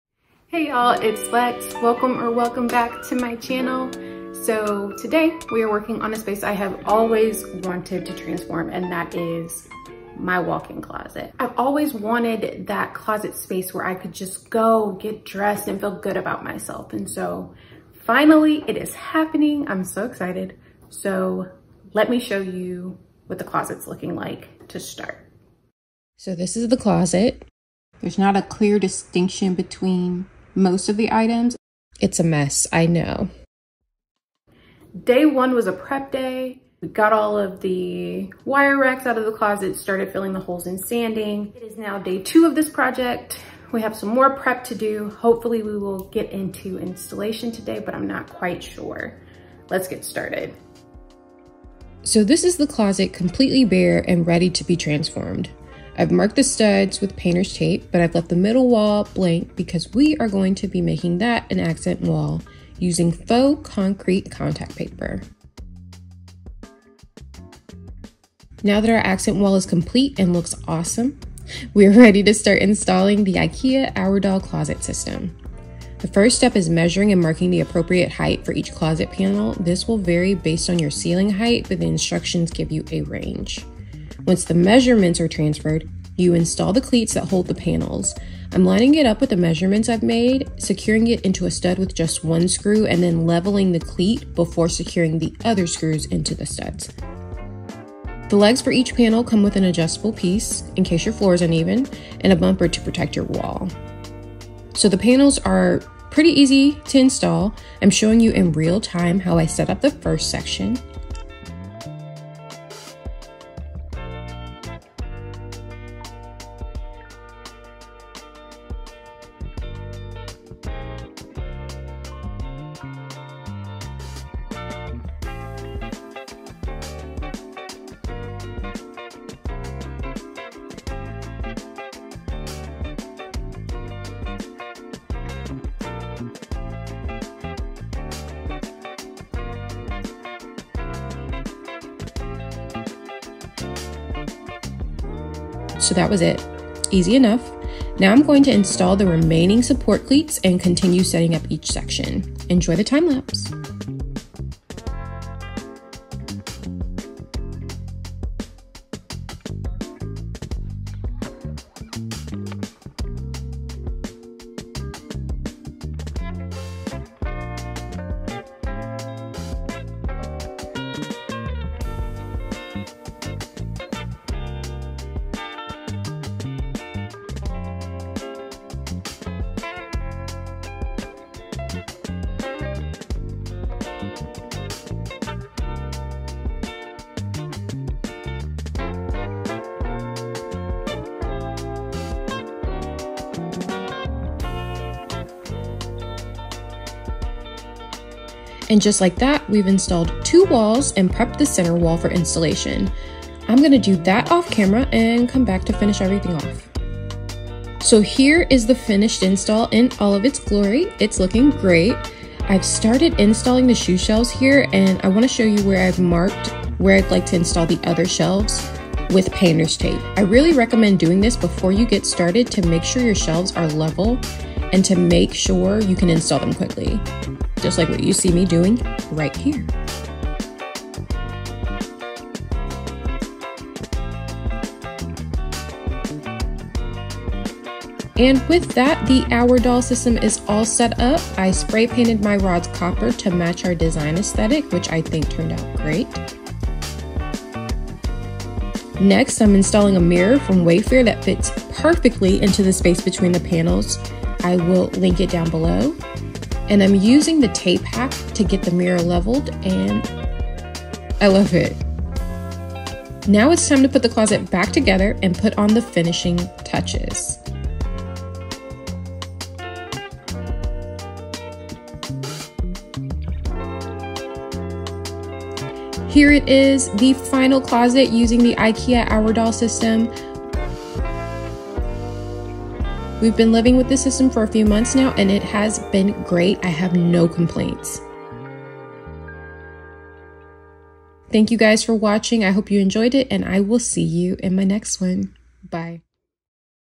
DIY Custom Closet | Master Closet Makeover | Ikea Aurdal Closet System | On a budget!
Hey y'all! This custom closet makeover using the Ikea Aurdal System cost only $1000.  Links are below.

Pinterest: DIY Diaries by Lex
Facebook: DIY Diaries by Lex

HOME DETAILS
3 beds 3.5 baths
2.5 stories
2 car garage